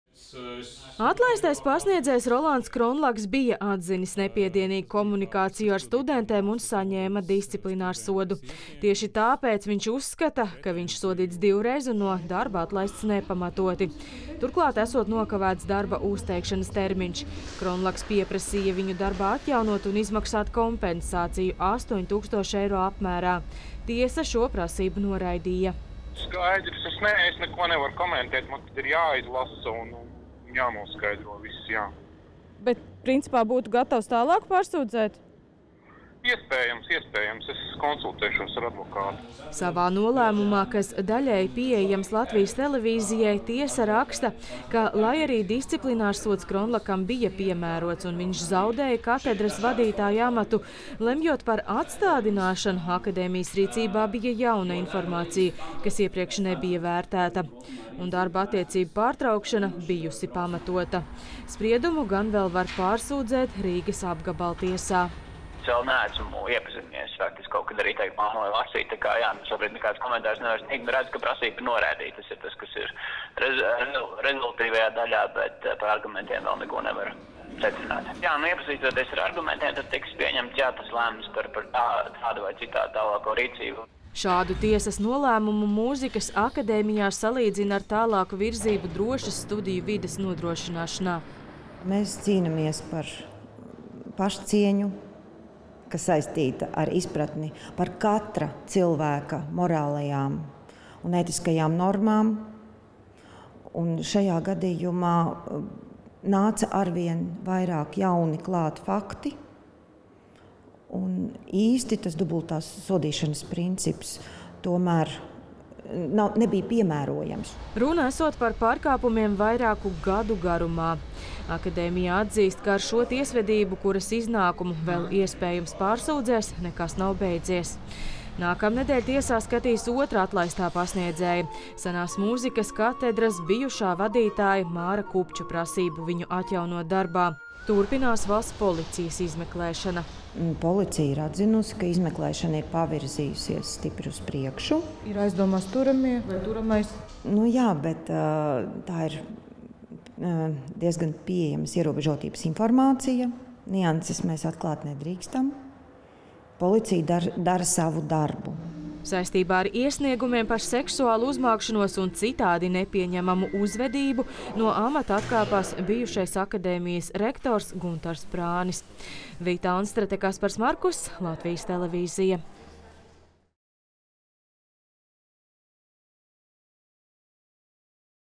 Noraida atlaistā pasniedzēja pieteikumu pret Mūzikas akadēmiju
Rīgas pilsētas tiesa šodien noraidījusi Jāzepa Vītola Latvijas Mūzikas akadēmijas Mūzikas tehnoloģiju katedras bijušā vadītāja Rolanda Kronlaka prasību tiesā pret savu bijušo darba devēju. Izskatot strīdu par Kronlaka atbrīvošanu no amata, tiesa norāda, ka akadēmija rīkojusies tiesiski. Līdz ar to noraidāma Kronlaka prasība par viņa atjaunošanu darbā un pieprasītā kompensācija 8000 eiro apmērā. Ar to gan tiesu darbi akadēmijai nebeigsies. Nākamnedēļ sāks skatīt lietu, kurā pret akadēmiju vērsies cits atlaistais darbinieks – Māris Kupčs.